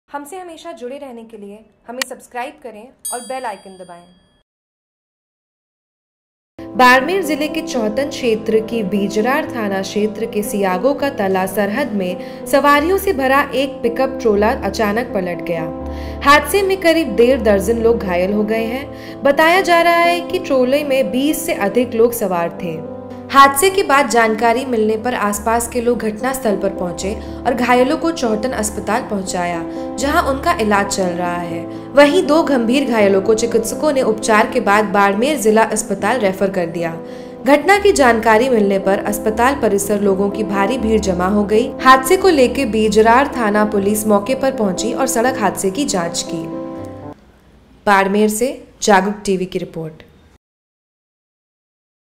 JAGRUK TV : अनियंत्रित पिकअप ट्रोला पलटा : डेढ़ दर्जन लोग घायल
बाड़मेर- जिले के चौहटन क्षेत्र के बीजराड़ थाना क्षेत्र के सियागों का तला सरहद में सवारियों से भरा एक पिकअप ट्रोला अचानक पलट गया। हादसे में करीब डेढ़ दर्जन लोग घायल हो गए। बताया कि रहा है कि ट्रोले में 20 से अधिक लोग सवार थे।